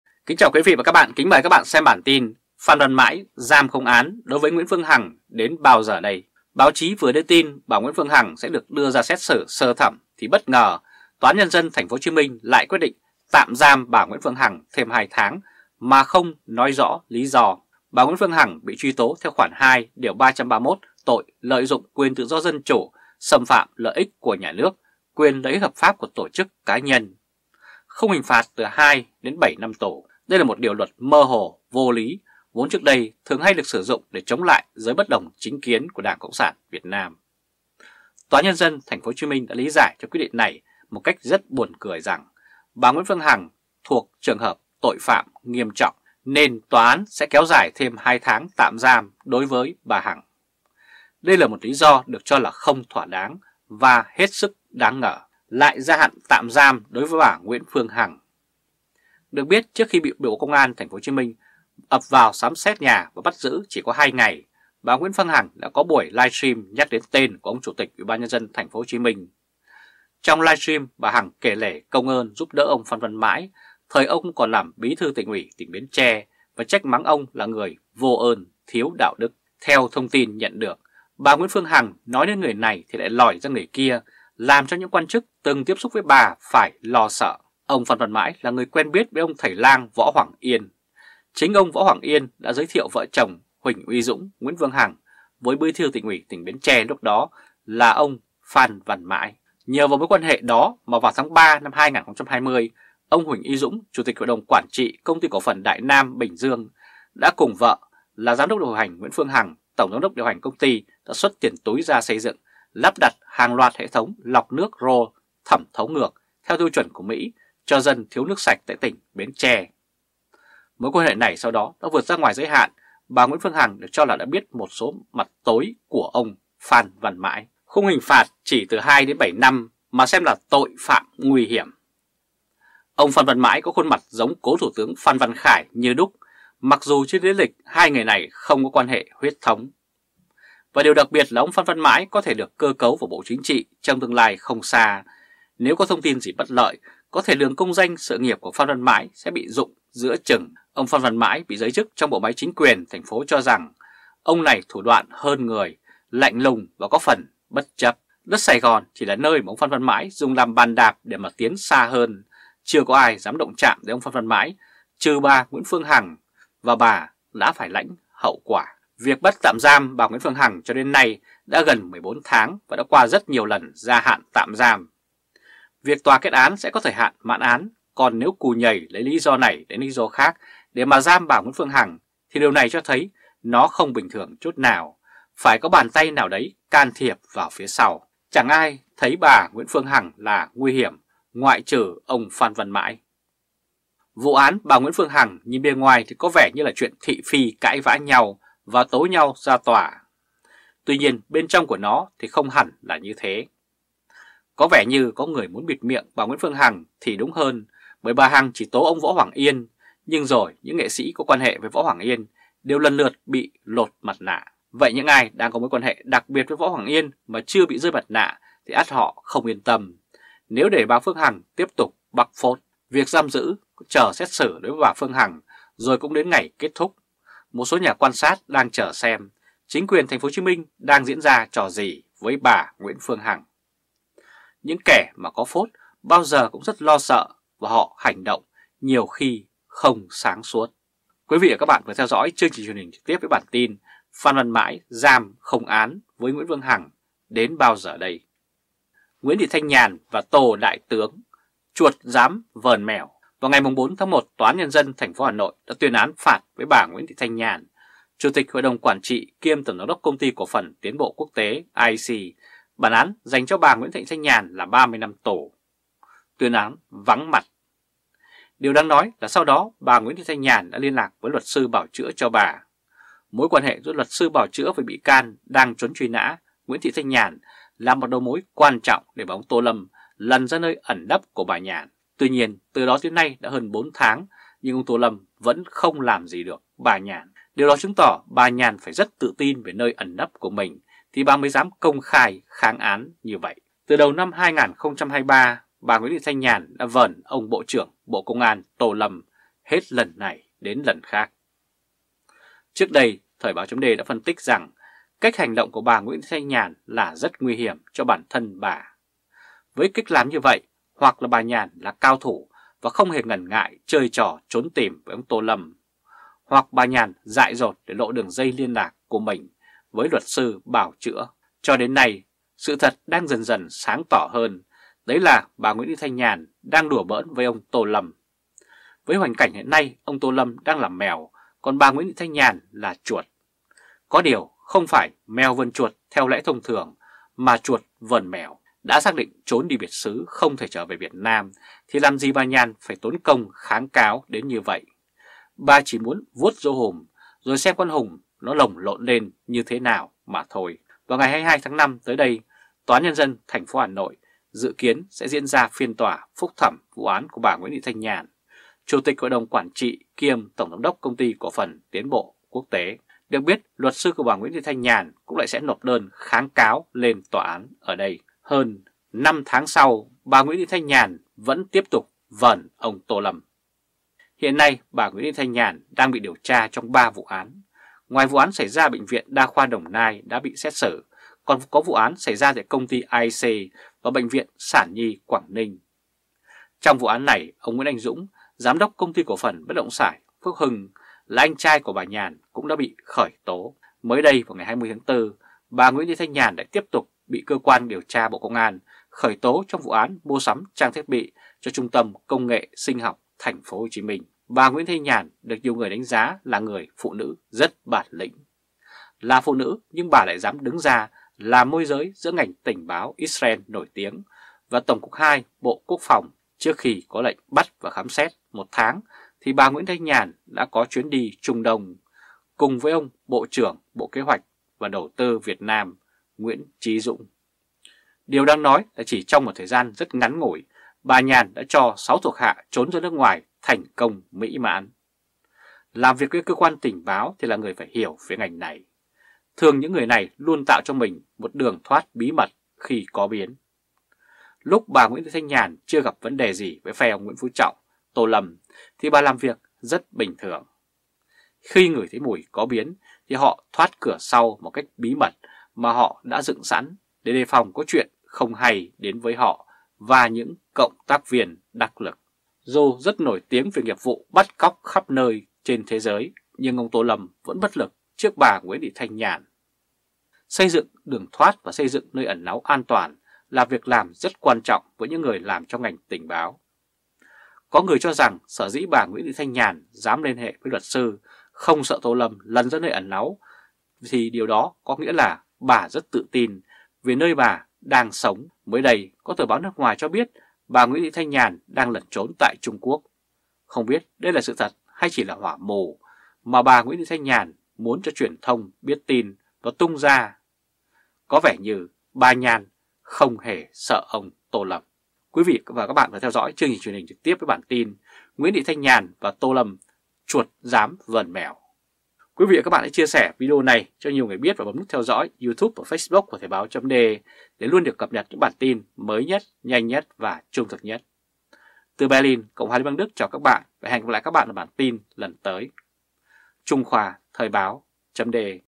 Phan Văn Mãi „giam không án“ đối với Nguyễn Phương Hằng đến bao giờ?
Việc giam giữ chờ xét xử đối với bà Phương Hằng rồi cũng đến ngày kết thúc. Một số nhà quan sát xem đang chờ xem, chính quyền TP. HCM đang diễn trò gì với bà Nguyễn Phương Hằng. Những kẻ mà có phốt, bao giờ cũng rất lo sợ và họ hành động nhiều khi không sáng suốt.